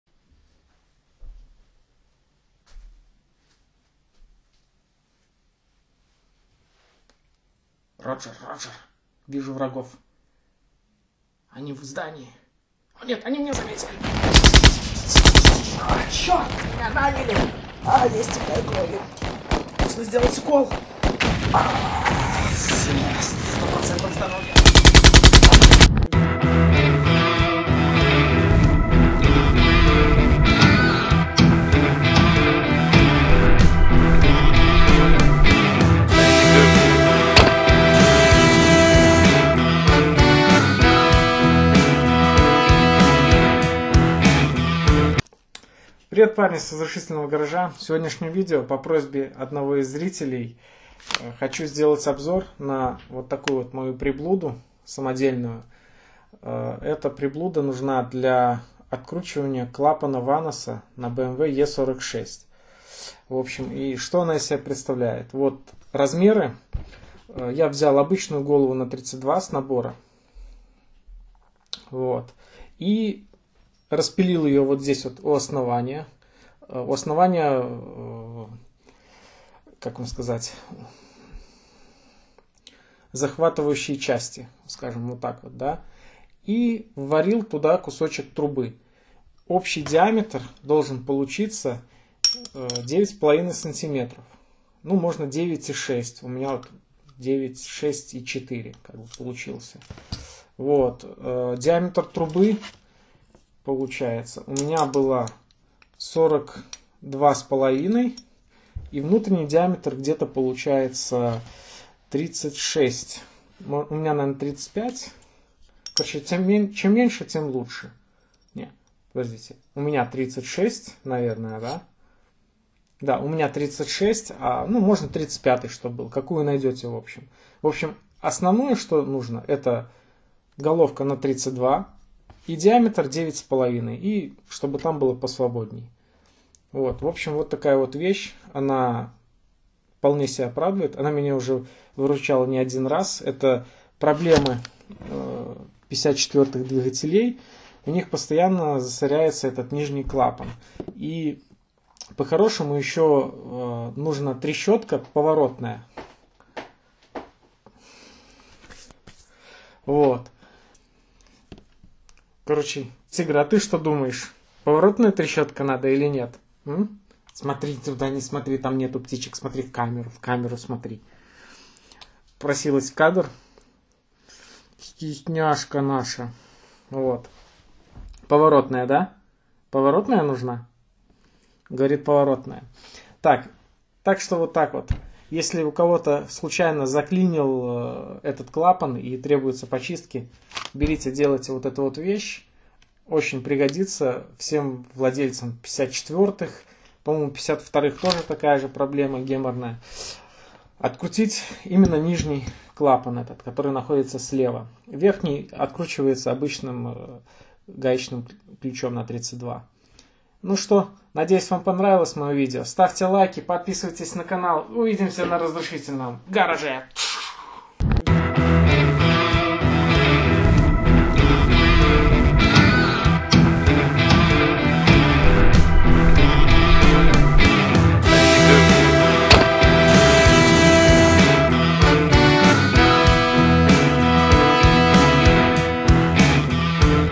Как открутить клапан ваноса. Спец инструмент для бмв е 46
спец инструмент для бмв е 46 или чем открутить клапан ваноса на м54 моторе, да головка на 32 должна быть шестигранная длинной 9.6 см., как открутить вот в этом ролике есть немного описание